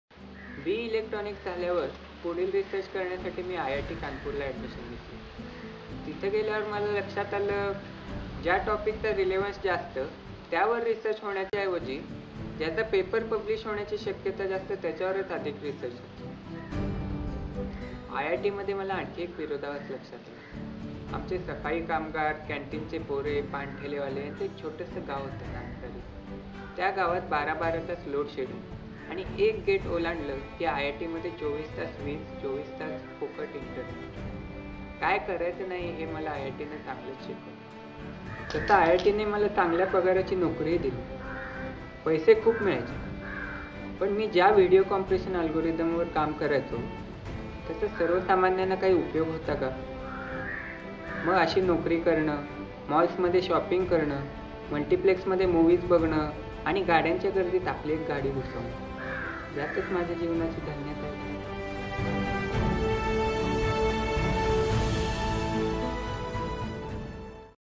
NIRMAN: Youth for purposeful life
IIT सारख्या नामांकित कॉलेज मधून शिक्षण घेवूनही सामाजिक क्षेत्रात उतरण्याचा निर्णय का घेतला ? या बद्दल सांगत आहे , निखील जोशी .. ..